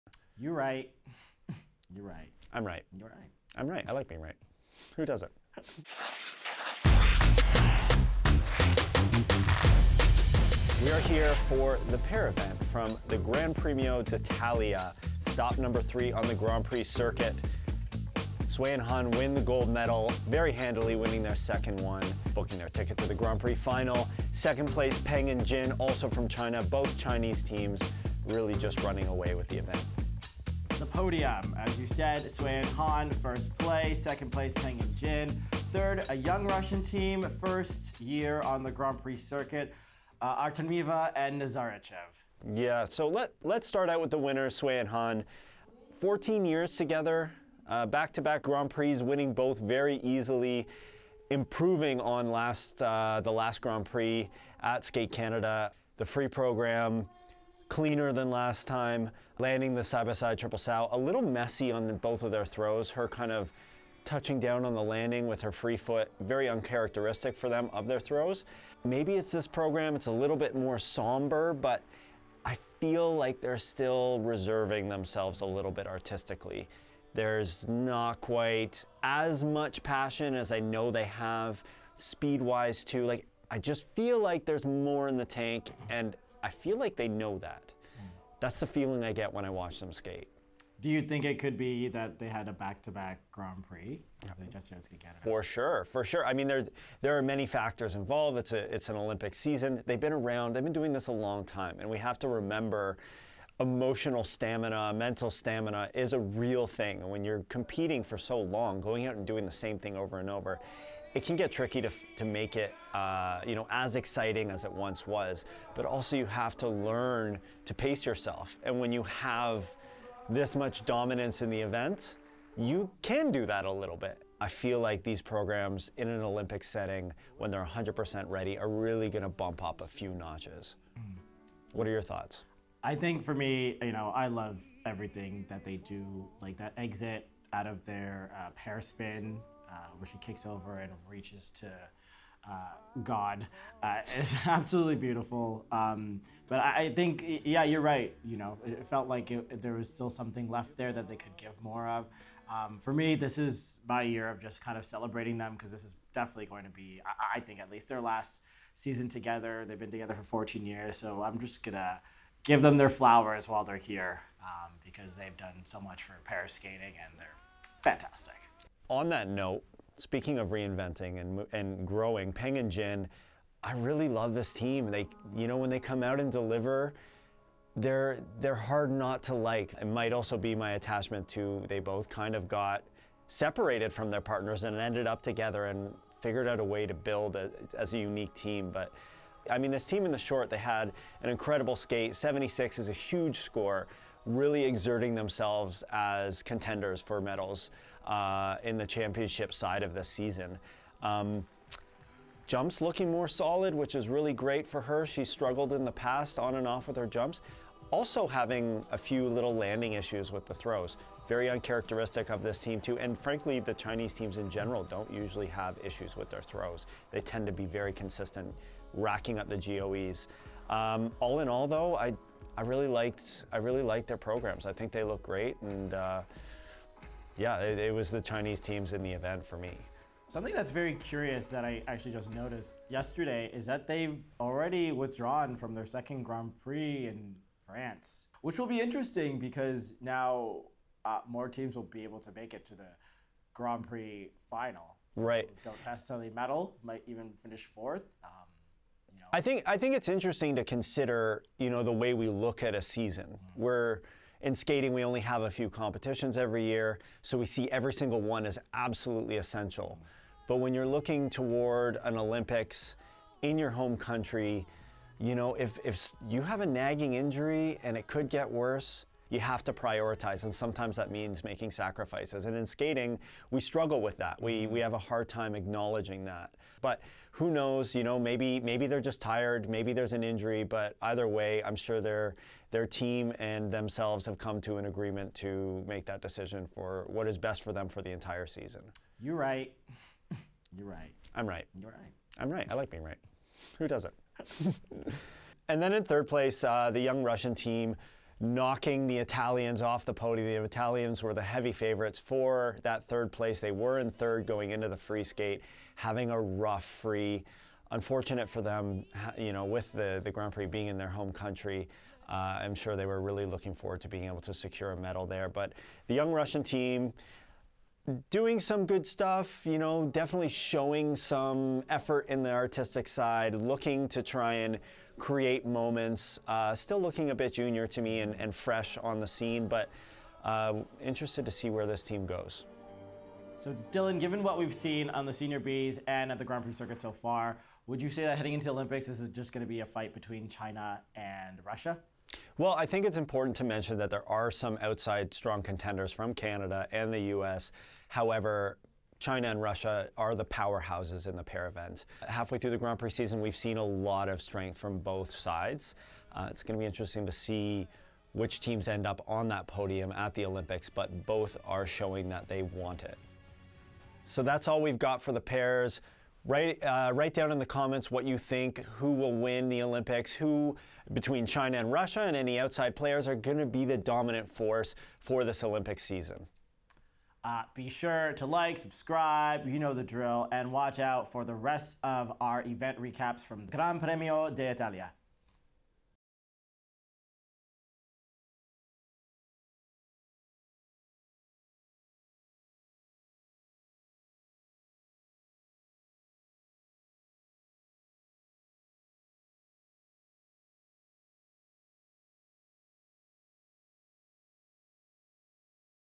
Sui & Han win gold in Italy, Russians Artemeva & Nazarychev make podium | THAT FIGURE SKATING SHOW
The Chinese dominated in the pairs event at the Gran Premio D'Italia. Wenjing Sui and Cong Han won their second gold medal in just two weeks, Peng Cheng & Yang Jin took silver, and Iuliia Artemeva & Mikhail Nazarychev leave their first senior Grand Prix event with bronze. It's looking like the Olympic pairs event in Beijing might be a competition between the Chinese and Russian teams. Do you think the Canadians or Americans can challenge for a podium spot?

0:00 Recapping the pairs event at Gran Premio D'Italia
00:42 Wenjing Sui & Cong Han win gold in Italy
2:48 Cheng Peng & Yang Jin take silver at Grand Prix
4:55 Iuliia Artemeva & Mikhail Nazarychev take bronze at Grand Prix debut
5:33 Will the Olympic pairs podium be China & Russia?